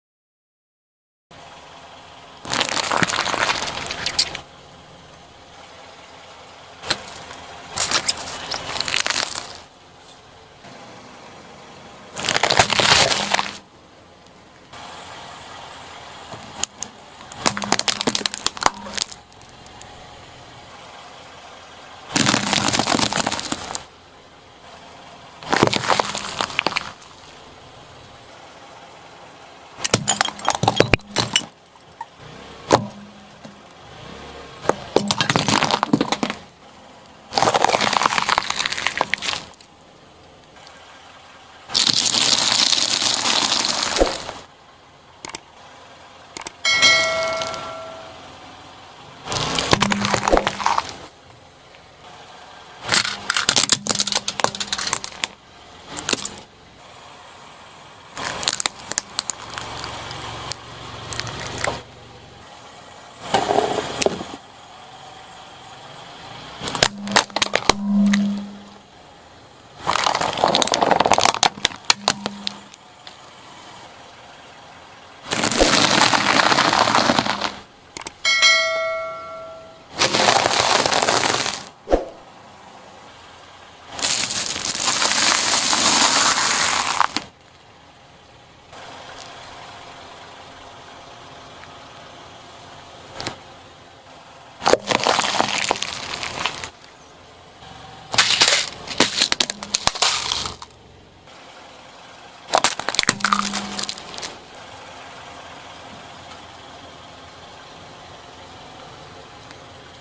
Crushing Crunchy & Soft Things by Car! - EXPERIMENT: CAR vs. HALLOWEEN
In this video, I'll show you what it's like to crush crunshy and smooth things with a car, in this experiment called CAR vs. HALLOWEEN !.

Which of these crunchy and smooth things will withstand after being crushed by my car. I love to smash things with my car!